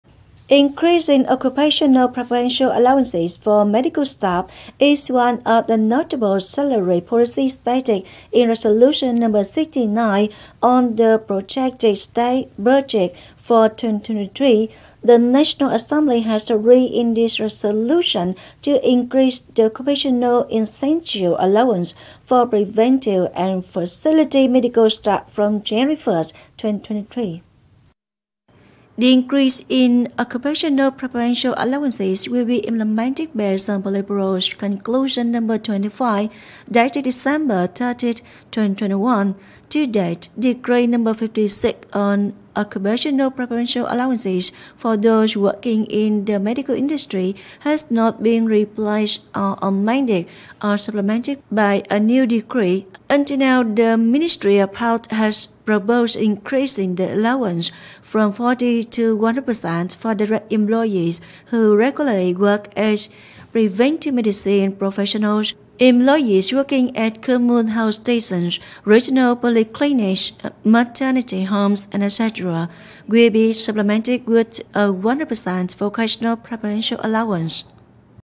Occupational preferential allowances for medical staff increased| Cần Thơ News
-Increasing occupational preferential allowances for medical staff is one of the notable salary policies stated in Resolution No.69 on the projected state budget for 2023. The National Assembly has agreed in this resolution to increase the occupational incentive allowance for preventive and facility medical staff from January 1, 2023.